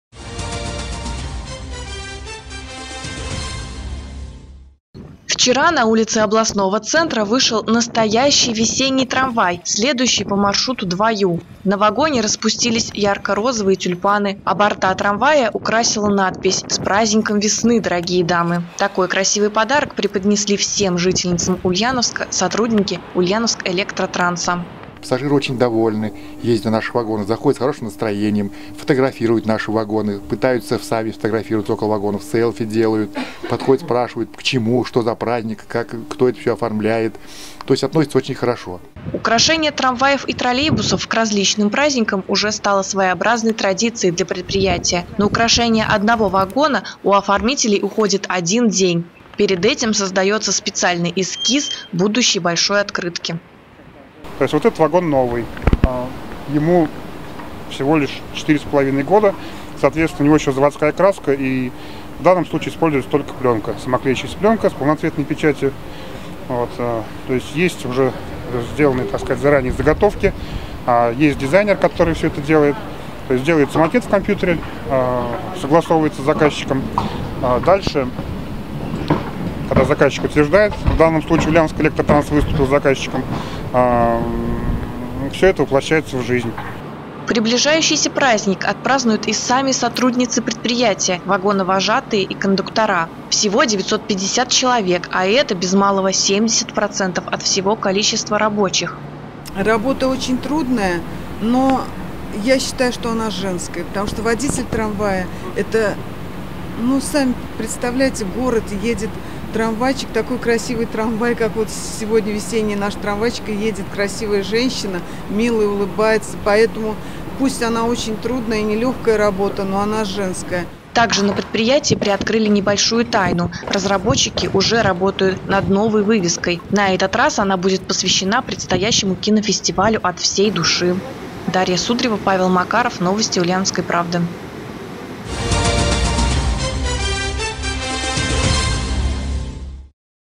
Трамвай к 8 марта
«Ульяновскэлектротранс» поздравил всех представительниц прекрасного пола необычным способом.